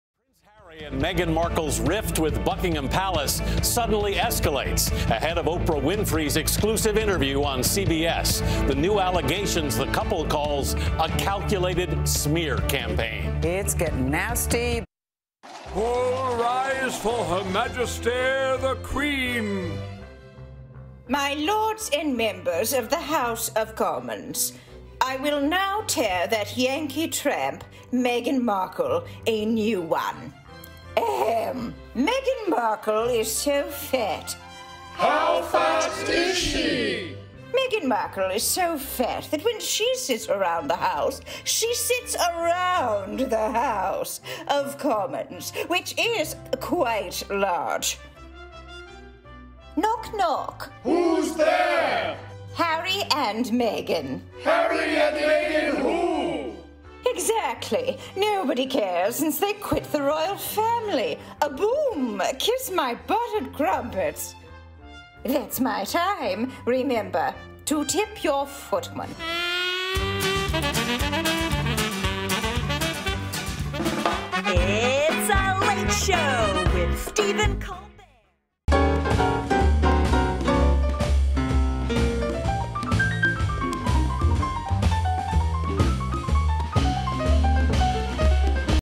The Queen's Got Jokes For Meghan Markle
When the Queen of England breaks out the Book of Slams, you know it's serious.